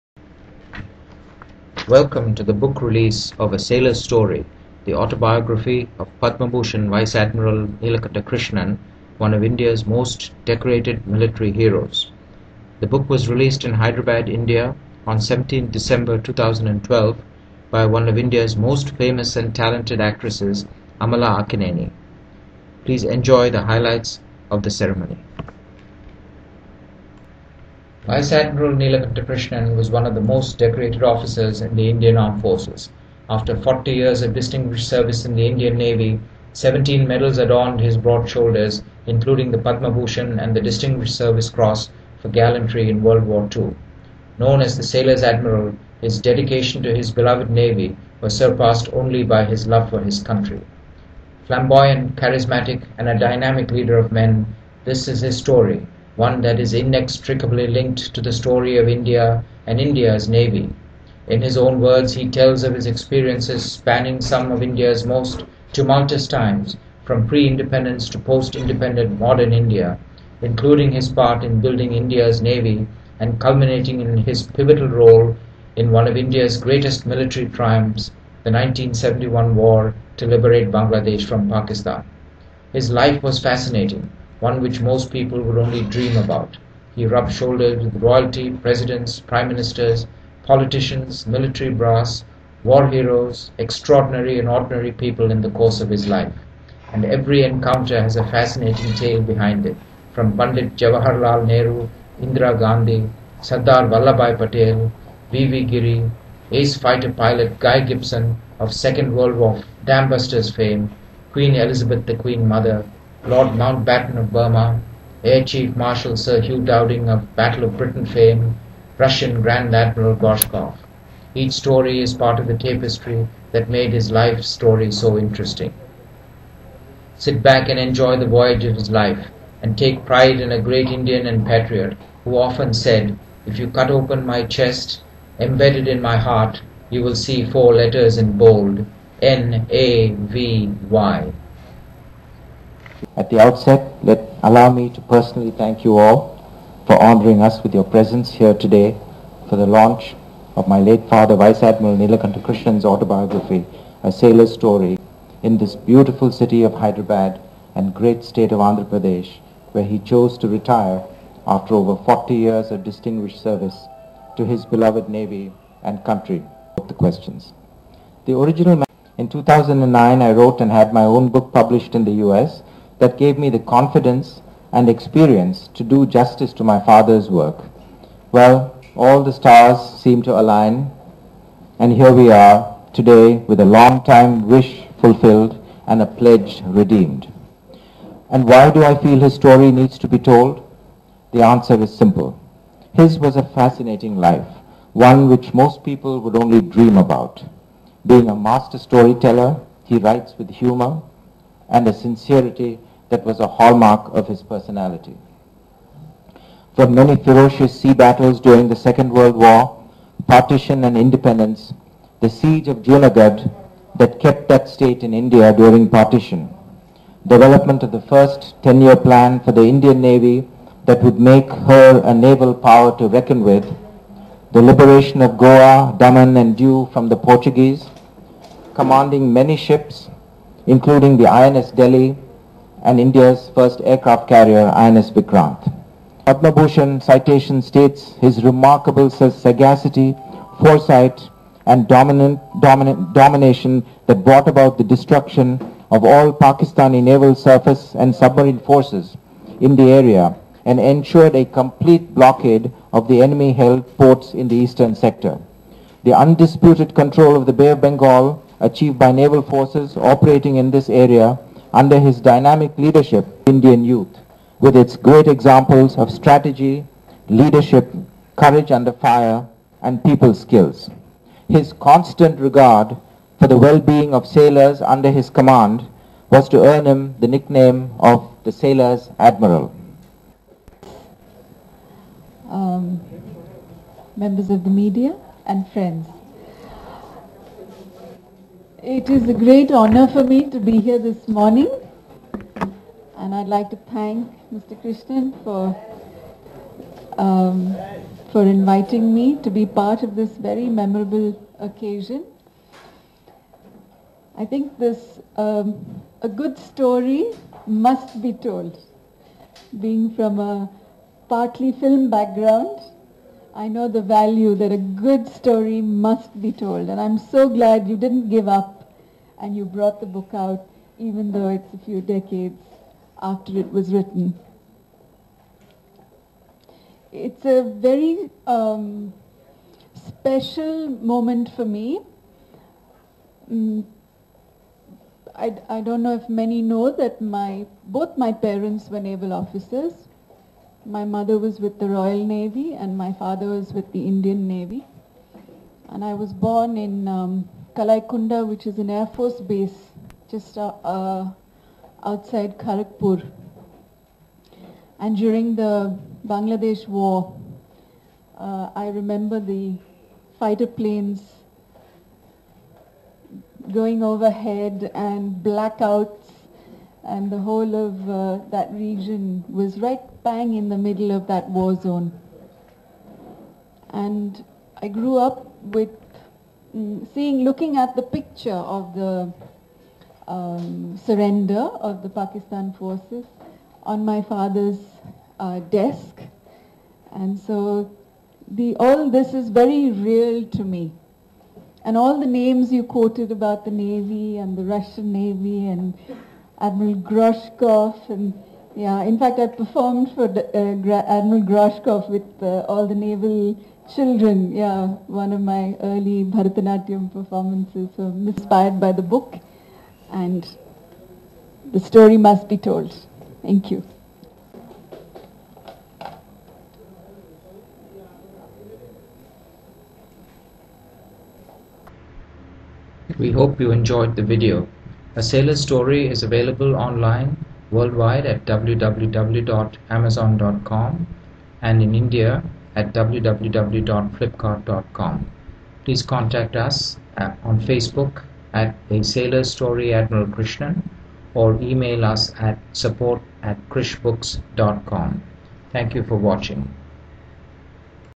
Vice Admiral N. Krishnan Autobiography A Sailors Story Book Launch Amala Akkineni & Arjun Krishnan
Amala launches A Sailors Story by Vice Admiral N. Krishnan, at Hyderabad, India on December 17th, 2012. The book is the autobiography of one of India's most decorated military heroes, Admiral Krishnan with 17 medals including the Distinguished Service Cross fir valor in the second world war, and Padma Bhushan for his pivotal role in the liberation of Bangladesh in December 1971, and surrender of all Pakistani forces in the Eastern theatre and the sinking of Pakistani submarine PNS Ghazi. The book has been edited and published by his Son Arjun Krishnan.